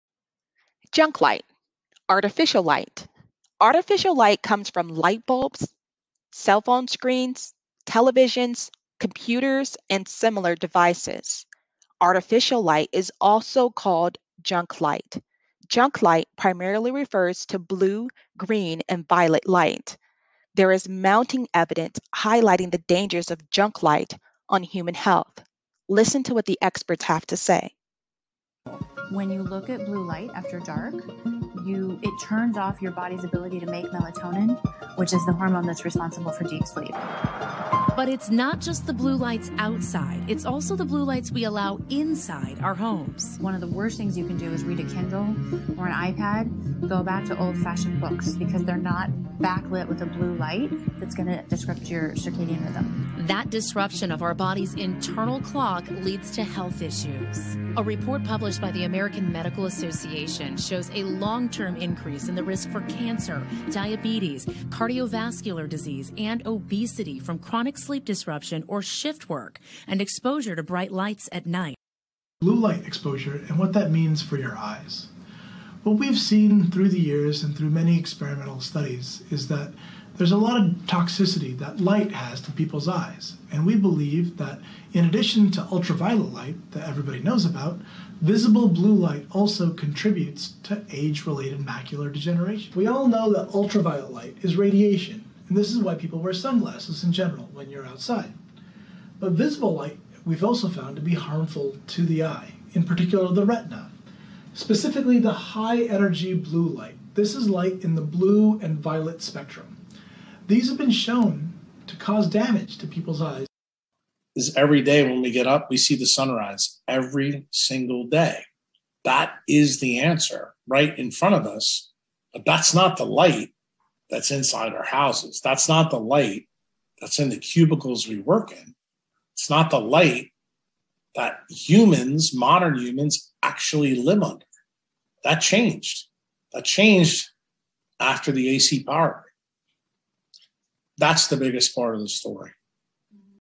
The Dangers of Artificial Light
This video covers the Artificial Light section in the 21 Day Assess Journal. This video also  describes how artificial light is bad for your health.